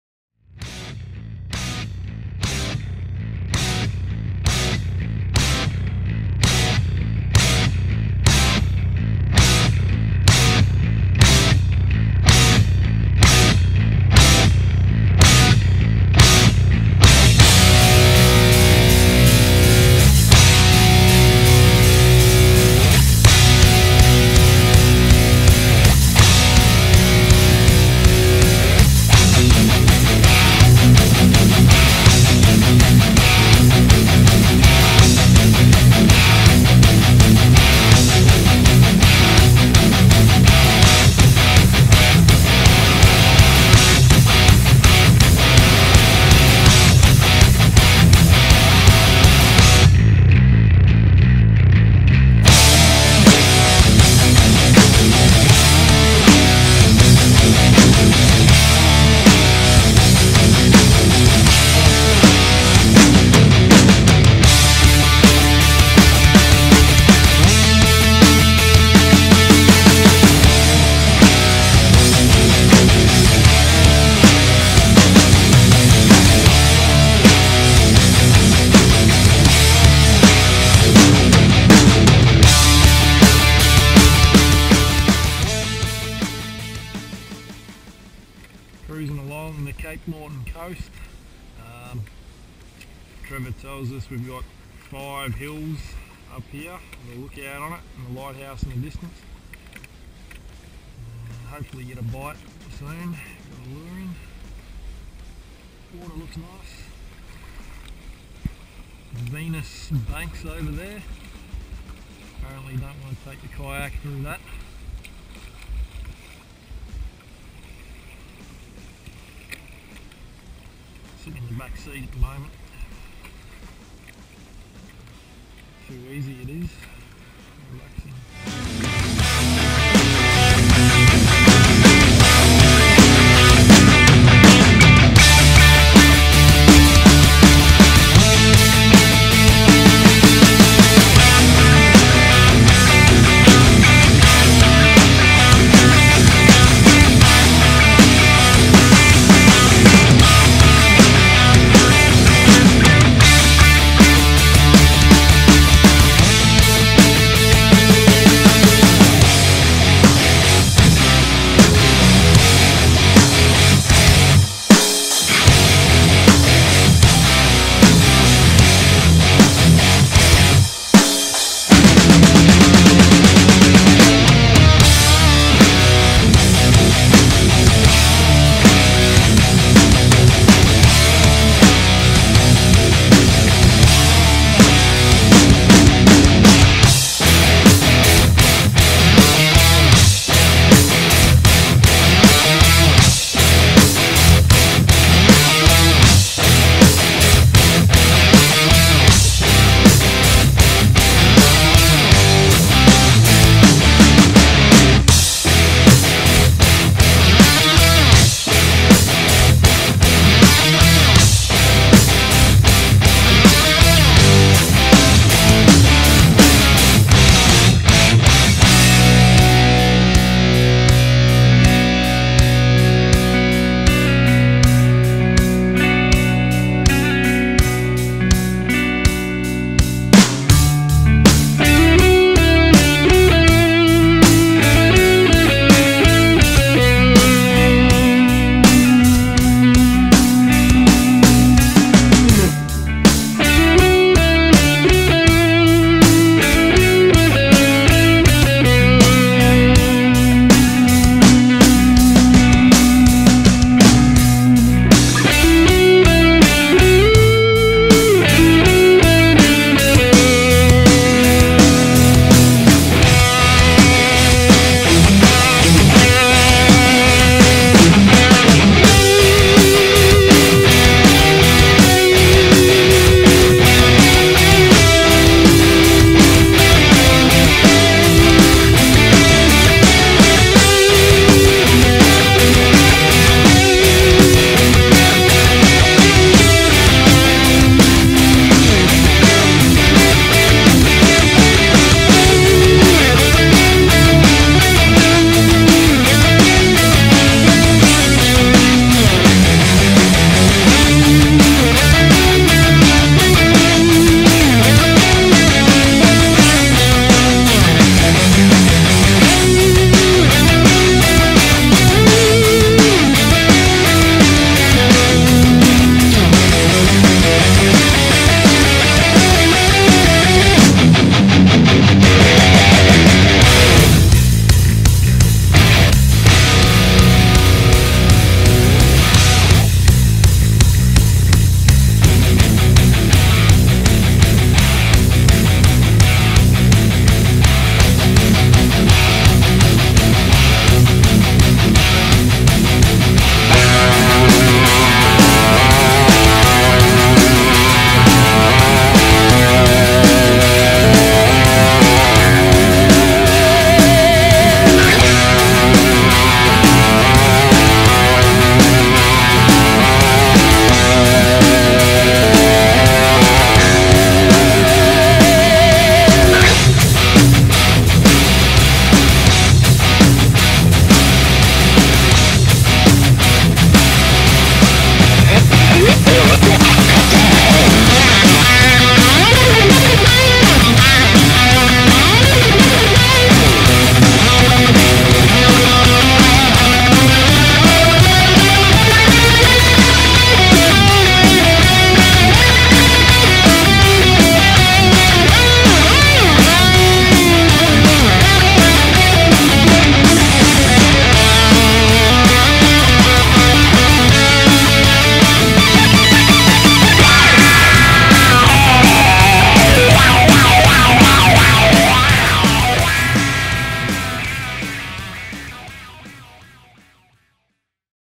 Cape Moreton - Hobie Tandem Island 2012
Sailed the kayaks to cape Moreton from Bribie with some strong winds coming back on the Sunday. We camped at the cape. No fish caught but a good trip.